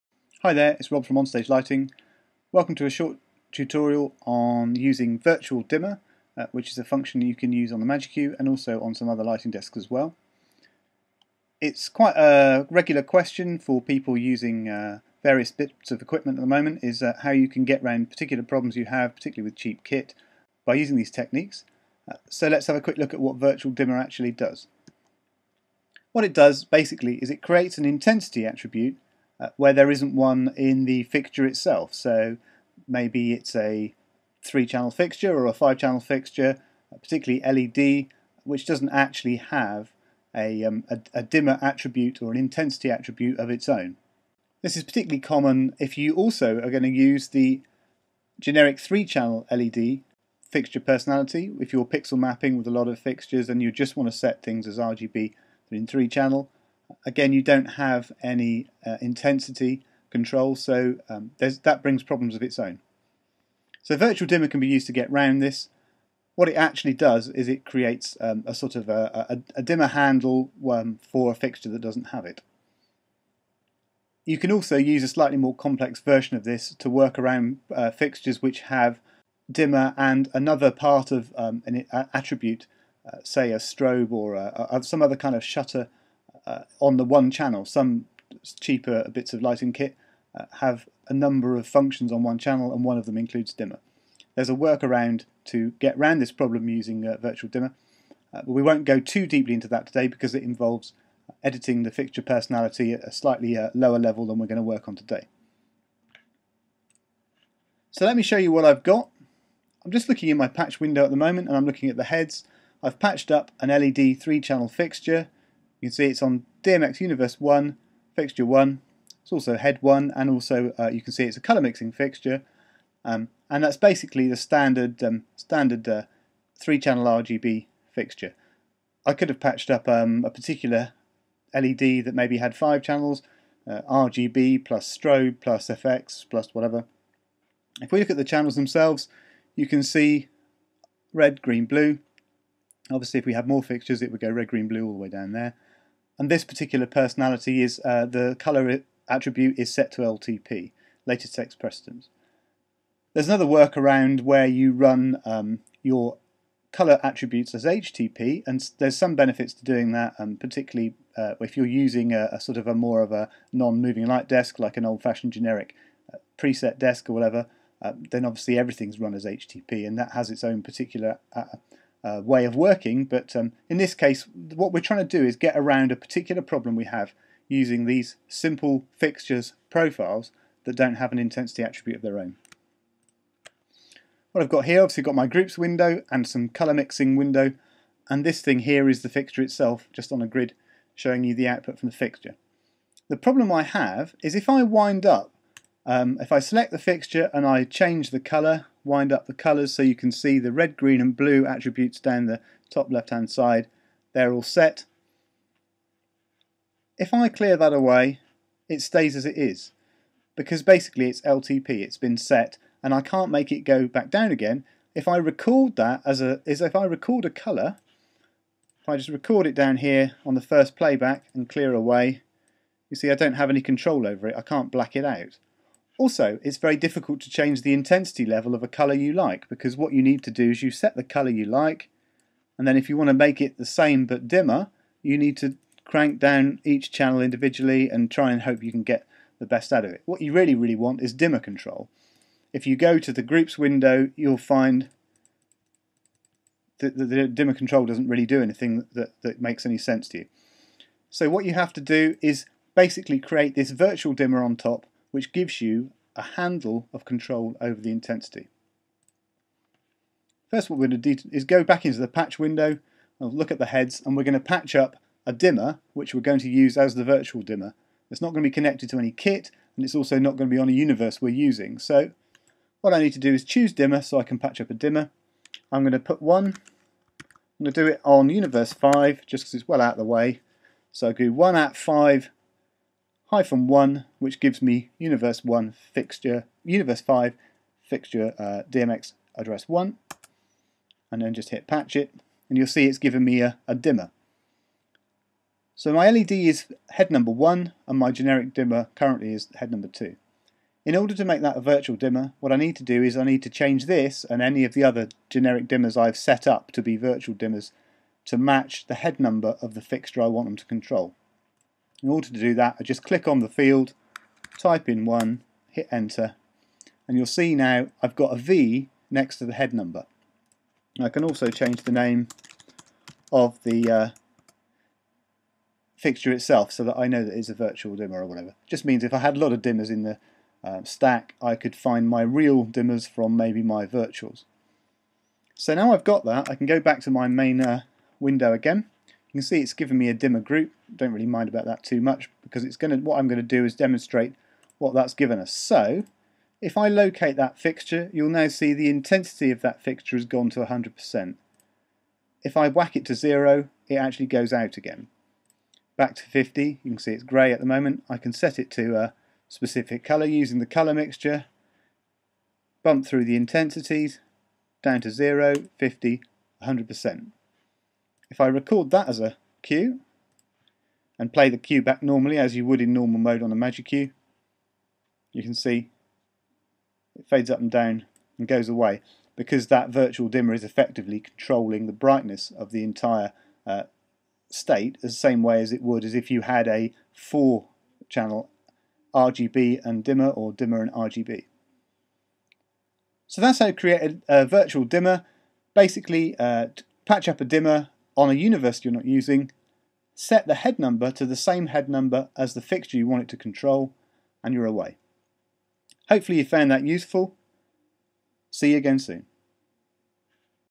MagicQ Virtual Dimmer Tutorial
UPDATE 2013:

PLEASE NOTE: Since software 1.5.6.0 Virtual Dimmer has changed in workflow AND in implementation on the MagicQ. This makes a lot of the video redundant and/or inaccurate.  I'll leave it here in case it helps someone.

From the MagicQ change log:

Virtual dimmers now take effect at the Patch level rather than at the Programmer level. This means that the Output Window, View Heads will show the virtual levels on the RGB channels (i.e. the RGB levels before the virtual dimmer is applied).

It is now possible to add virtual dimmers to a head in the Patch, View Heads view using a single action. Place the cursor on the head you wish to add a virtual dimmer for and then press SHIFT and ADD VDIM. You can use SHIFT + the cursor keys to select multiple heads.

Short tutorial on using Virtual Dimmer function on the Cham Sys MagicQ and some situations that it can be useful.
This tutorial covers:
Potential problems with particular LED and disco market lighting fixtures
Fixture personalities and how to change fixture attributes to LTP or HTP
Patching a Dimmer and setting up Virtual Dimmer